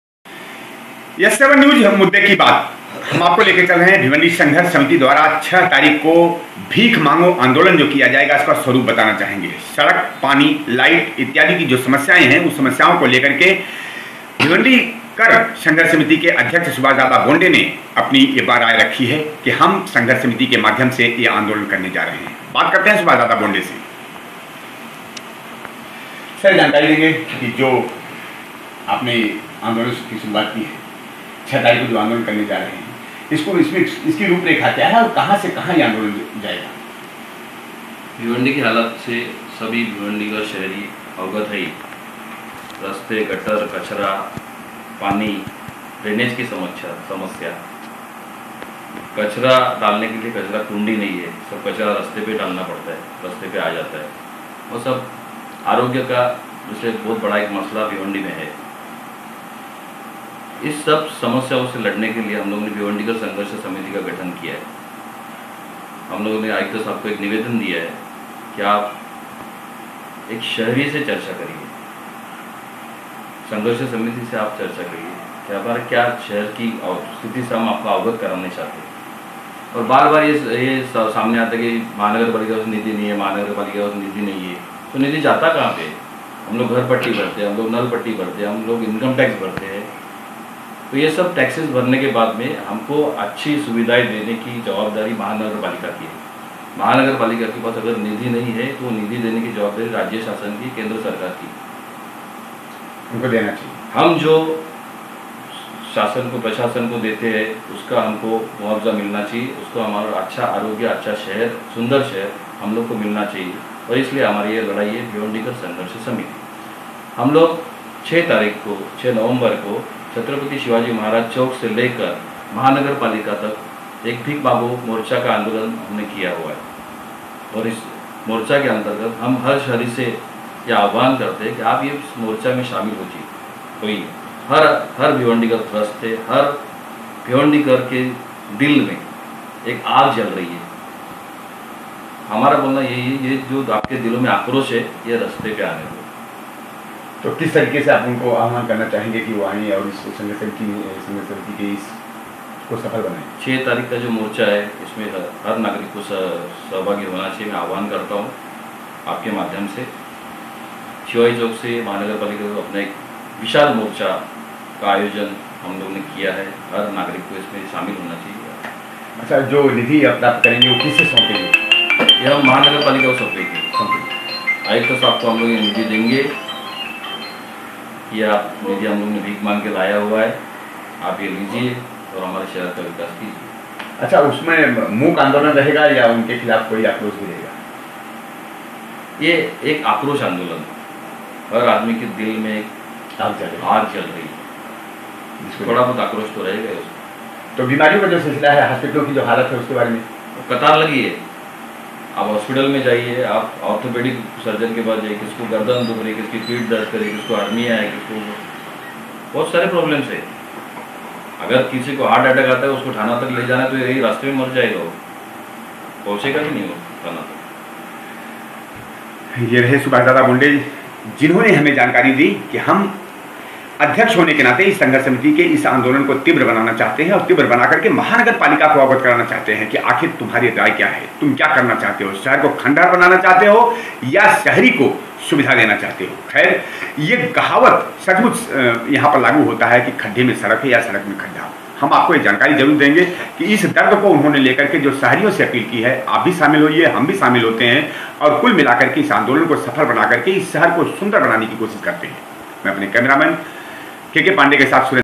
मनपा के लिए माँगे भीख - भिवंडी कर संघर्ष समिती
बेहाल भिवंडी शहर की सड़क और अन्य समस्या से परेशान भिवंडी वाशियो के लिए भिवंडी कर संघर्ष समिति का भीख मांगो आंदोलन ६ नवम्बर